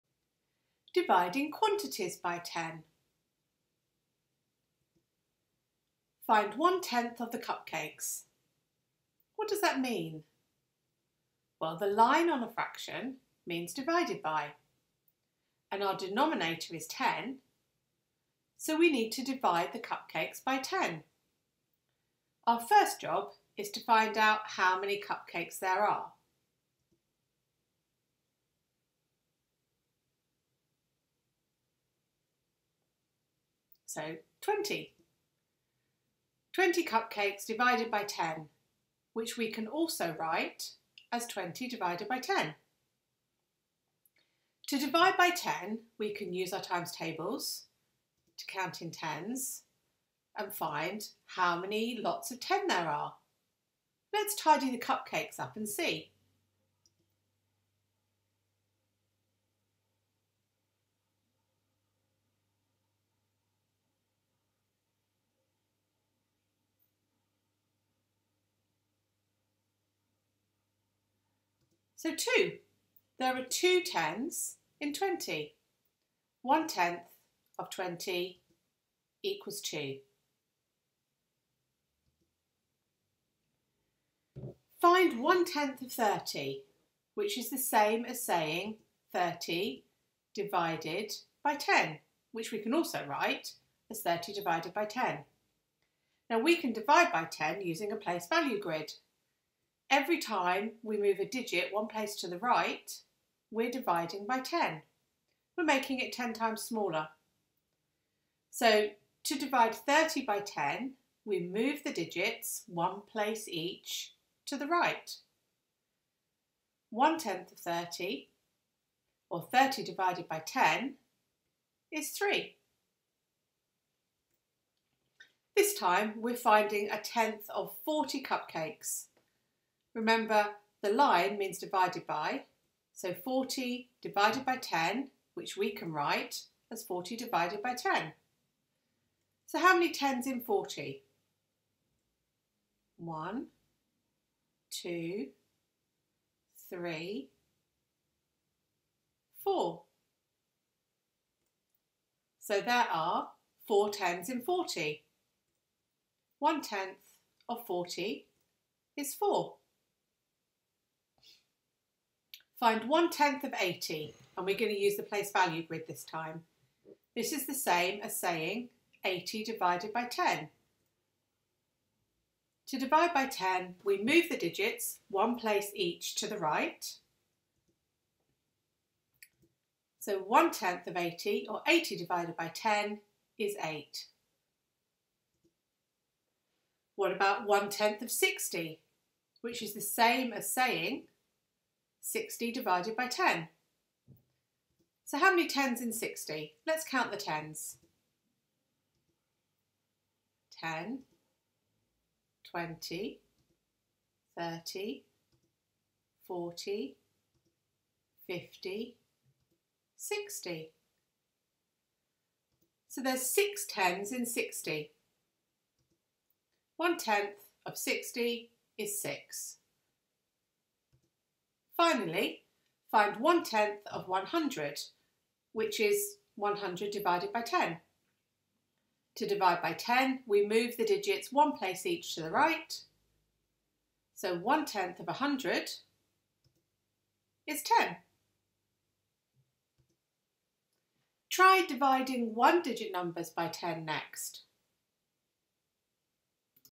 How to find a tenth of quantities. Year 3. Fractions, decimals, tenths. National Curriculum, KS2.
This video covers finding a tenth of quantities by dividing.  This is year 3 fractions from the UK National Curriculum.

Fractions, Year 3, National Curriculum - Pupils should be taught to recognise that tenths arise from dividing quantities by 10.